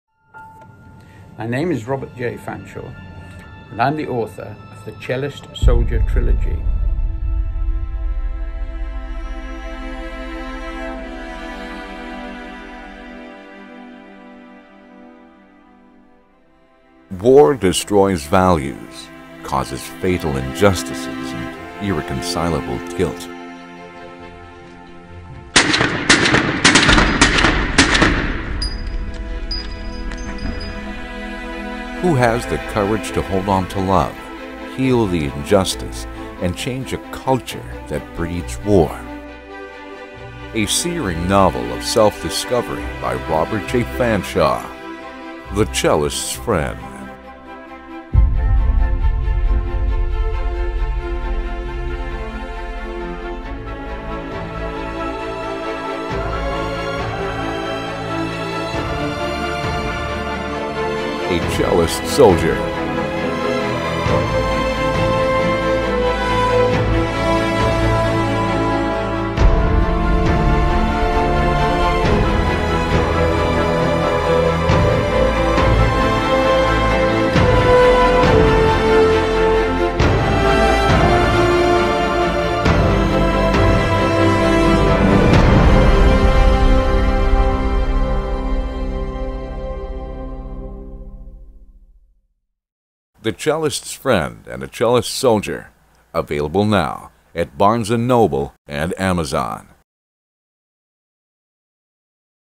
The Cellist Soldier Trilogy
A Cellist Soldier by Robert Fanshawe – Released on Amazon as Sensational and Gripping Story Inspired by True Events.

This story has the power to captivate different levels of audience. It has the warmness of a true hero blood persona whose life line is hanging on a thread on the events of war. Robert Fanshawe has captured a very detailed and grit war time capsule and delivered it in this book. As astonishing as the author, the book has an unforgettable quotes and lines which promises to deliver vast emotions to the reader.